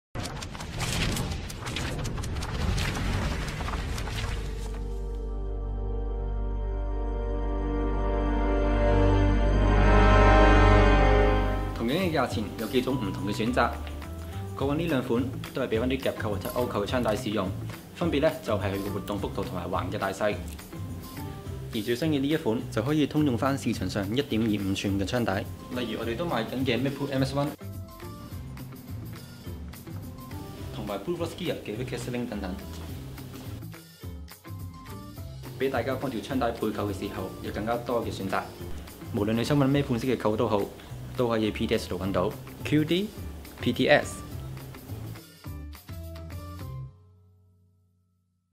想搵啱心水嘅 QD 扣？ | 槍帶配件 | 廣東話【pts-steelshop.com產品介紹】
最新 PTS 1” QD Sling Swivel 依家已經接受預訂，而PTS Steel Shop都有幾種唔同款式供各位槍迷選擇，同樣地適用於市面上嘅槍帶，價錢亦屬類近等級，買之前先聽聽 Steel Shop 同事對產品簡單介紹! QD? PTS!

Address:
Room 2602-05,
Wealth Commercial Centre,
48 Kwong Wa Street, Mongkok,